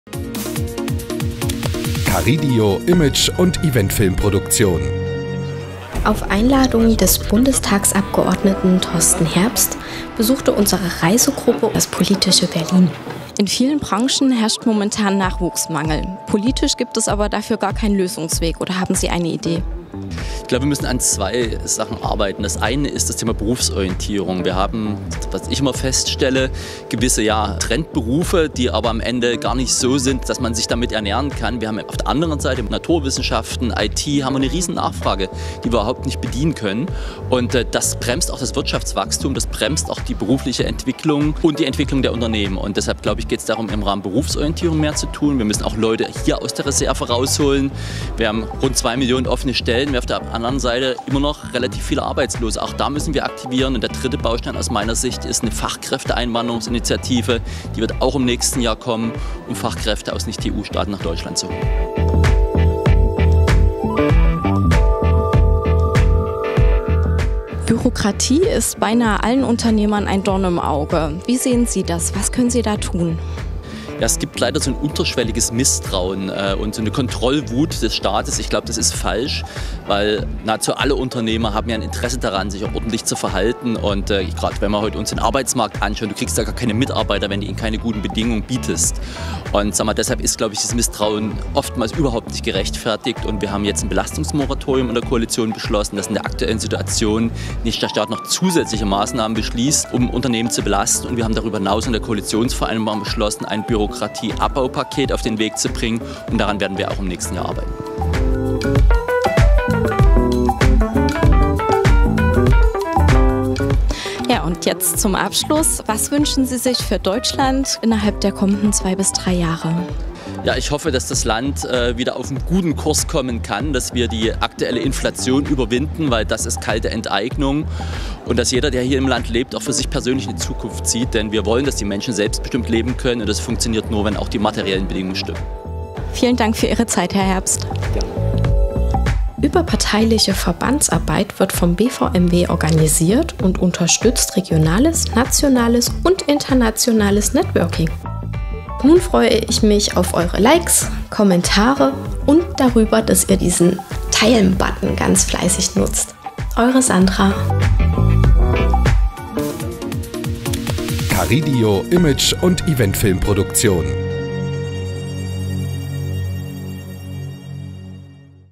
Teil 2 Interview Video Torsten Herbst (MdB) - Bundestagsabgeordneter für Sachsen - sehr informativ
Teil 2 Interview Video mit Torsten Herbst (MdB) - Unser Land neu denken - im Gespräch mit Sandra Wötzel / Sandras Social Media Creation, Redaktion und Moderation vor den Kameras der Karrideo Imagefilm-Produktion Torsten Herbst MdB über sich: Parlamentarischer Geschäftsführer der FDP-Bundestagsfraktion + Mitglied im Bundesvorstand der FDP + Landesschatzmeister der FDP Sachsen Dafür setze ich mich ein: Deutschland braucht ein Update - mit mehr Freiheit, bester Bildung, echtem Fortschrittsgeist, wirtschaftlicher Vernunft und einem starken Rechtsstaat. - Mobilität - Unser Land braucht eine Modernisierung der Verkehrsinfrastruktur. Dazu gehören die Elektrifizierung der Bahnstrecken Dresden-Görlitz und Leipzig-Chemnitz für einen schnellen Schienenverkehr sowie der zügige Ausbau von Bundesstraßen, zum Beispiel nach Zittau (B178 n) und Torgau (B87 n) ... u.v.m. - Recht - Die Zuwanderung in unser Land muss klar und eindeutig geregelt werden, wobei zwischen Asyl, Flüchtlingsschutz und Einwanderung zu unterscheiden ist. Von einem Einwanderungsgesetz nach kanadischem Vorbild würde nicht nur Deutschland, sondern auch Sachsen profitieren. ... u.v.m. - Einwanderung - Bei Einwanderern suchen wir uns aus, wer zu uns kommt. Für Kriegsflüchtlinge brauchen wir vorübergehenden humanitären Schutz, aber auch funktionierende Rückführungsregelungen nach dem Krieg. … u.v.m.

Bilder und Eventfilm von © Karrideo Image- und Eventfilmproduktion - Wir arbeiten deutschlandweit für Ihr Image und Ihre Events.